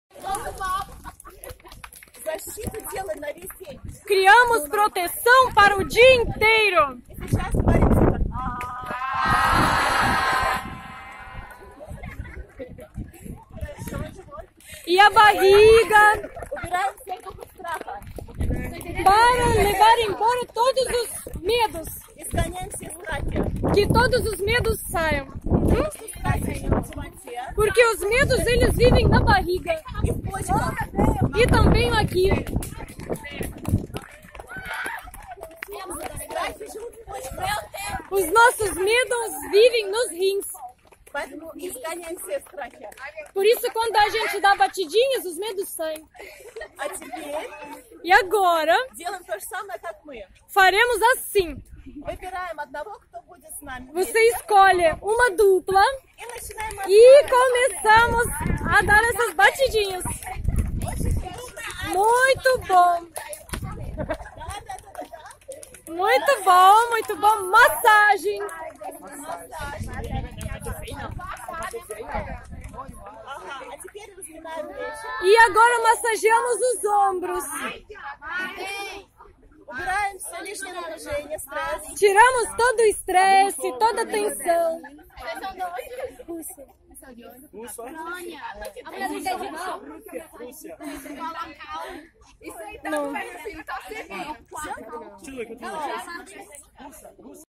Feliz manhã pratica para capacitar e proteger a nós mesmos
Muita alegria e felicidade durante todo o dia! 😍🙏💖Hoje em Fortaleza no Coco Park. Feliz manhã pratica para capacitar e proteger a nós mesmos. Os jovens são os mais abertos à felicidade e ao poder de transformação